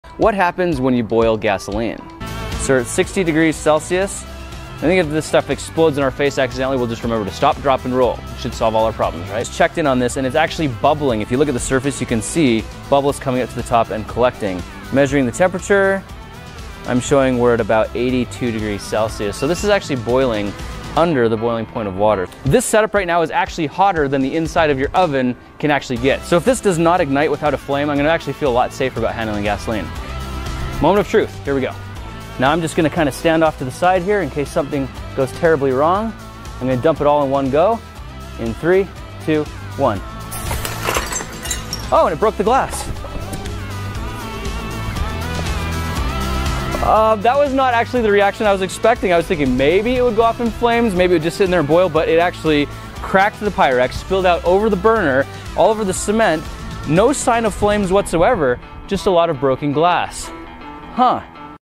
SHOULD YOU BOIL GASOLINE!?
This is what happens if you were to try and boil gasoline? Would it start a fire or would nothing happen at all?

Check out the list of materials we used, linked below! ⬇️🔗